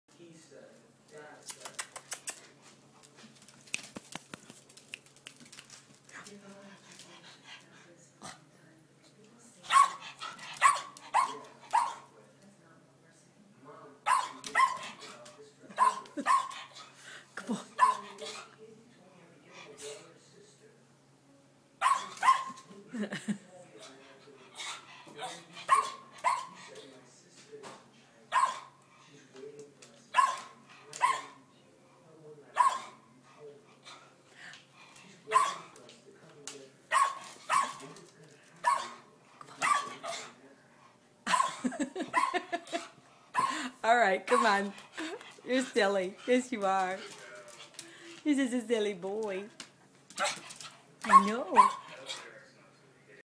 Joey the rescued devocalized Sheltie
Joey the rescued Sheltie playing. He has a wonderful, playful personality. - Adopted